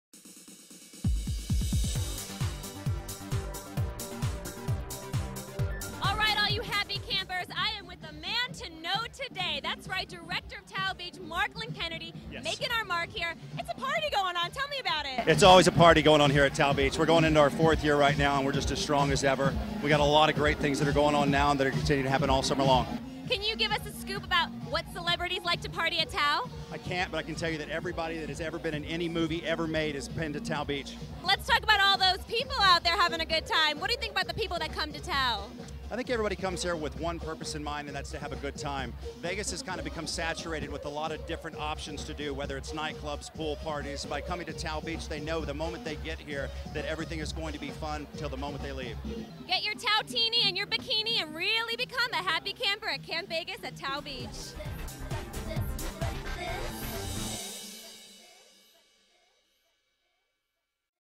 Marklen Kennedy TAO Beach Interview
Camp Vegas Counselor Shamanda chats up Marklen Kennedy, Director of TAO Beach about one of the Strip's hottest pools. Over the pulsating beats of the DJ, Marklen shares why the Vegas pool scene isn't to be missed.